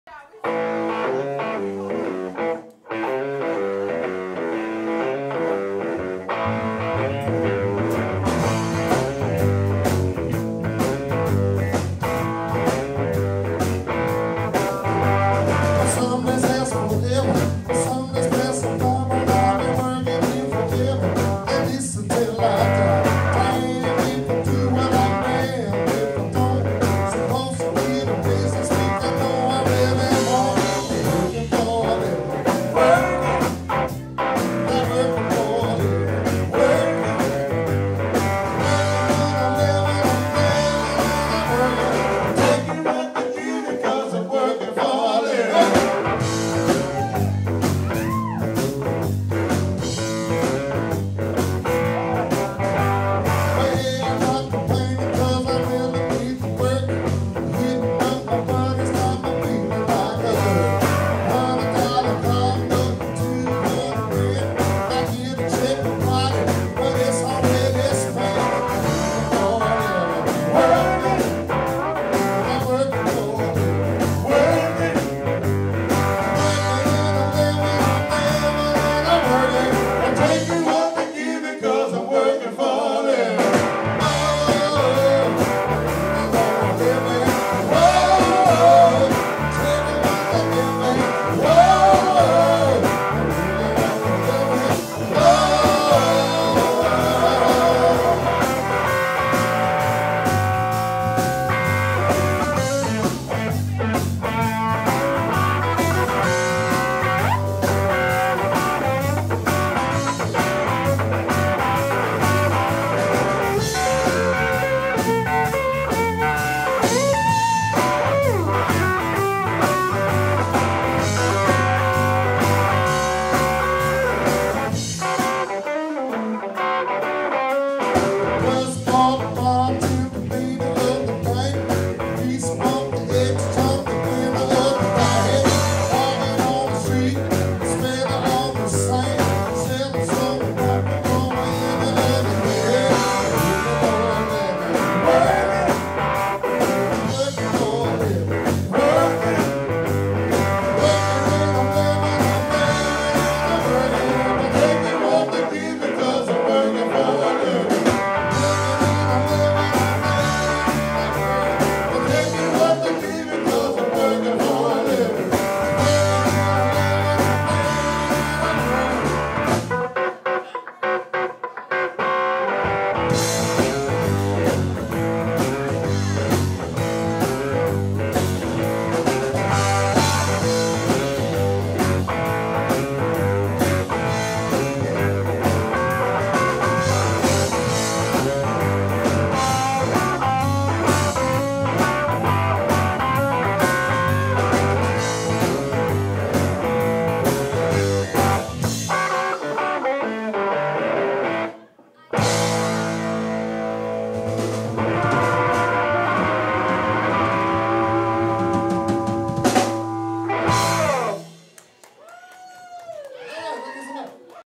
Ricky Jackson Performs at Katie's in Bacliff - 1:5:2013.MOV
Tonight we dropped by Katie's in Bacliff to see Ricky Jackson.